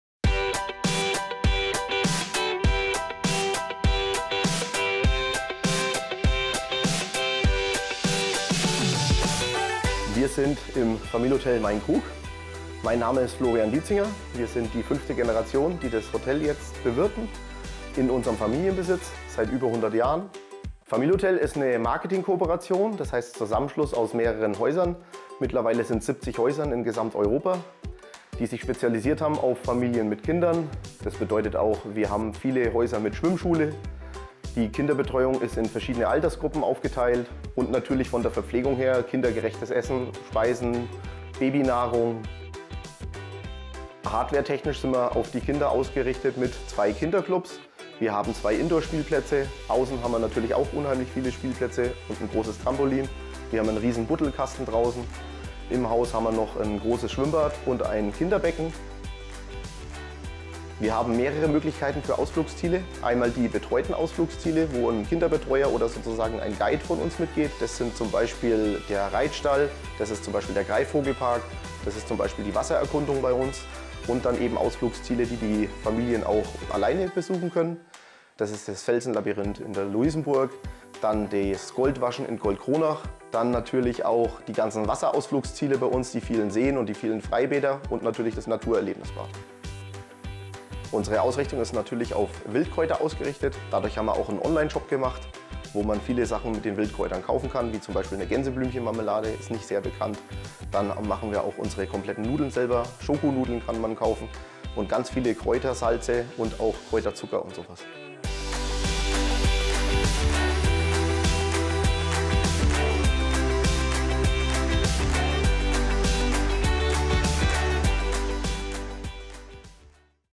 Seit 5 Generationen: Familotel Mein Krug im Fichtelgebirge – Familienurlaub mit Tradition!
Im Familotel Mein Krug im Fichtelgebirge genießen Familien seit 5 Generationen einen unvergesslichen Urlaub. In diesem Video zeigt dir der Inhaber die Geschichte des Hotels und wie es über die Jahre hinweg zu einem beliebten Ziel für Familienurlaub geworden ist. Vom historischen Hotelcharme bis hin zu modernen Annehmlichkeiten – hier kommt die ganze Familie auf ihre Kosten!

👀 Was du im Video sehen wirst:

Die Familiengeschichte von Mein Krug
Erlebnisse im Hotel und Umgebung
Warum das Familotel seit 5 Generationen so beliebt ist

🤳🏽Teile uns deine Gedanken in den Kommentaren mit und like👍🏽das Video!

🧳Bereit für deinen nächsten Familienurlaub?